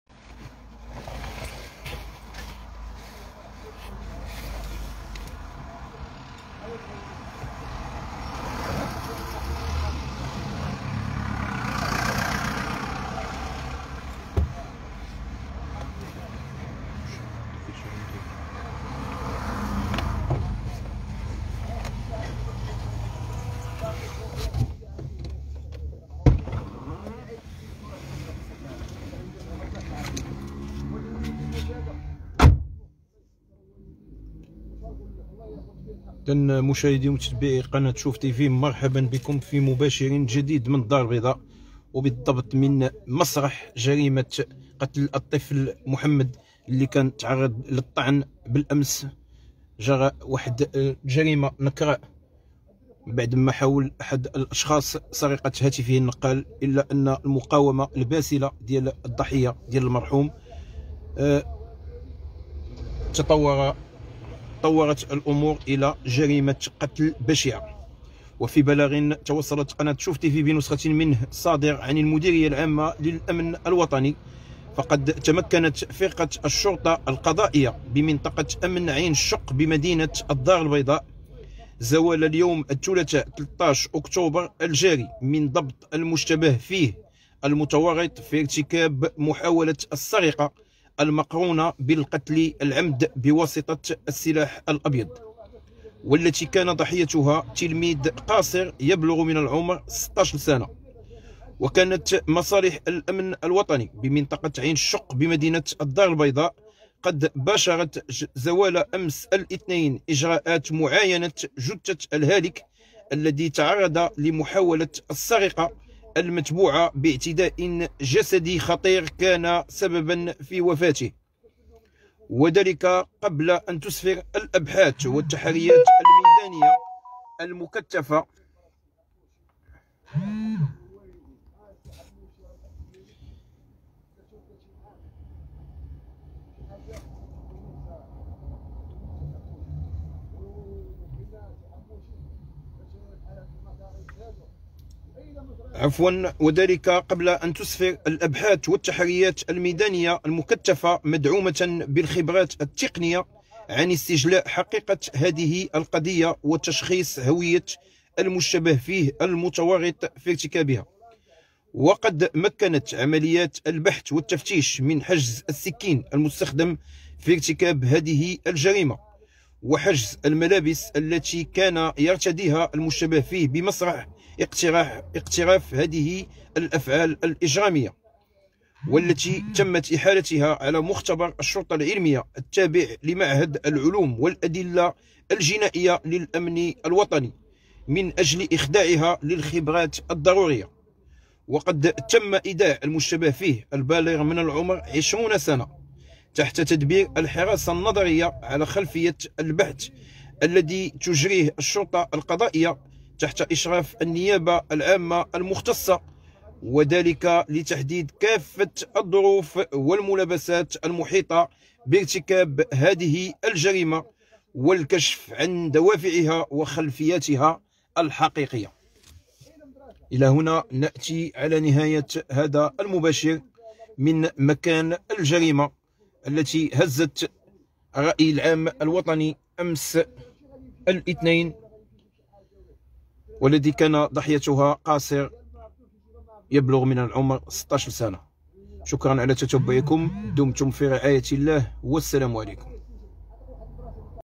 من الدار البيضاء..مديرية الحموشي تكشف التفاصيل الكاملة لاعتقال "مول البيكالا" لي قتل تلميذ وسط كازا
مباشرة من الدار البيضاء..مديرية الحموشي تكشف التفاصيل الكاملة لاعتقال "مول البيكالا" لي قتل تلميذ وسط كازا